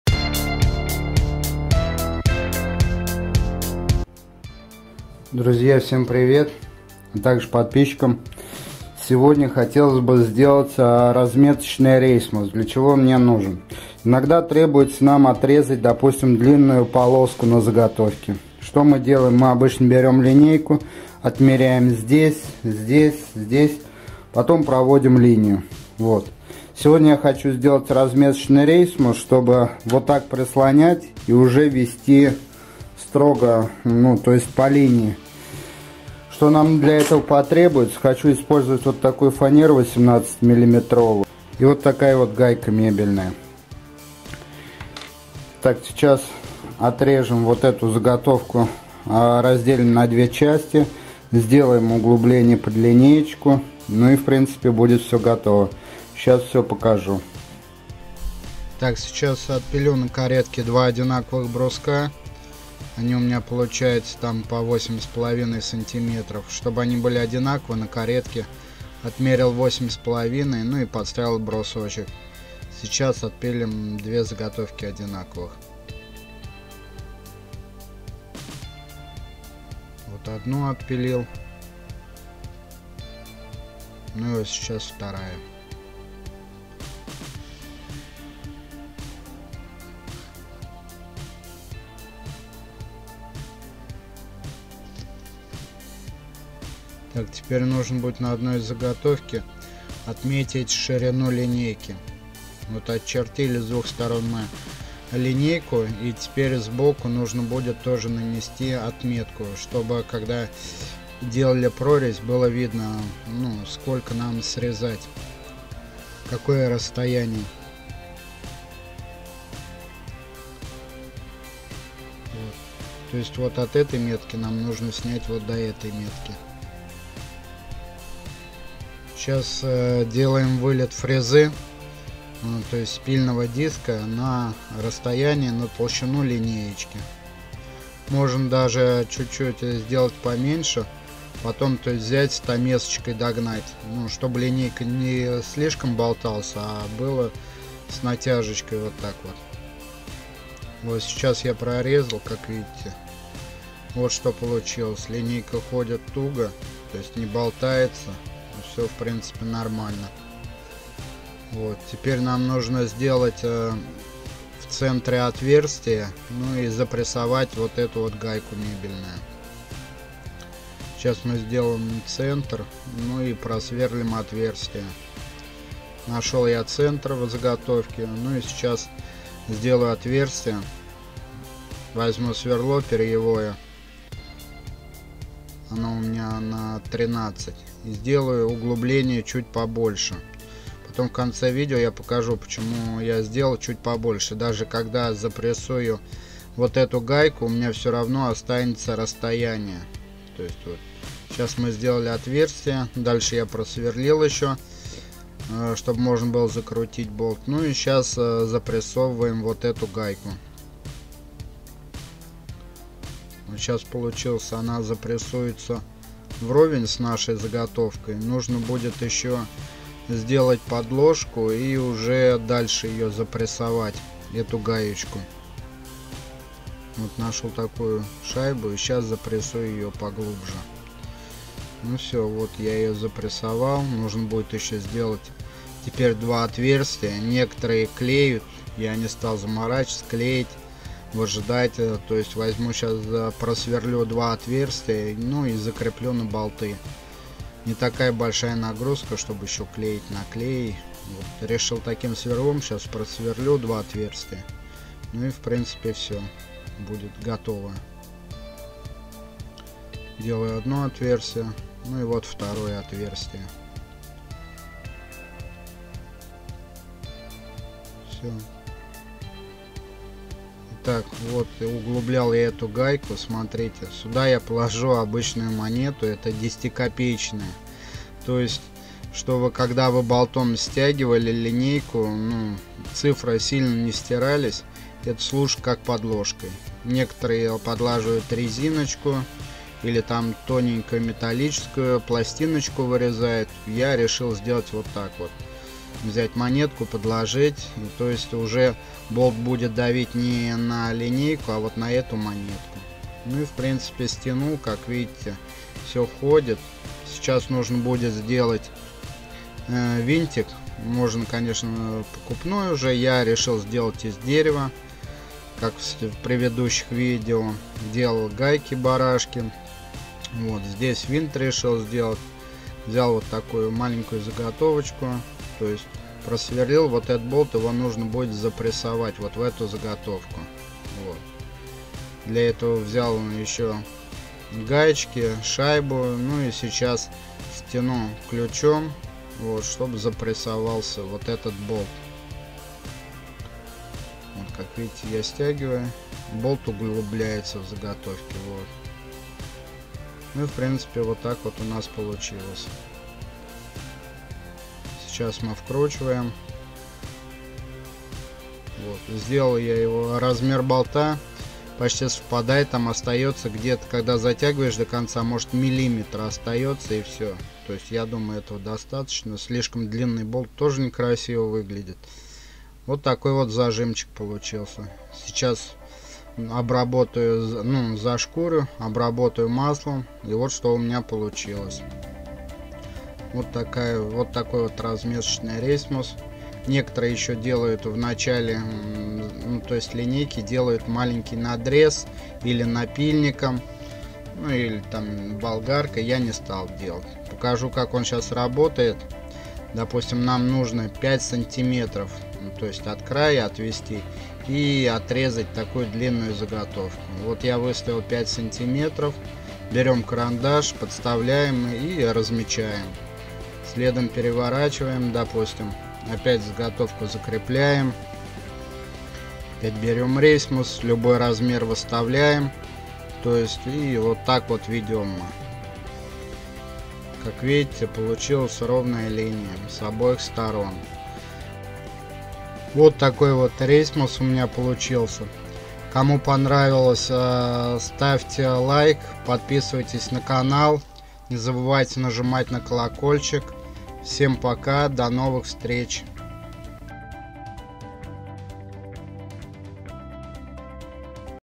МЕГАПОЛЕЗНАЯ самоделка! Щас всё покажу и расскажу!
Крутая идея для самоделки из обычного бруска! Рейсмус своими руками в домашних условиях!
ХИТРОСТИ СТАРЫХ МАСТЕРОВ! Лайфхак идея в домашних условиях! Секреты мастеров!
Полезные советы, идеи, хитрости старых мастеров, Станки и приспособления, самоделки, ждут тебя!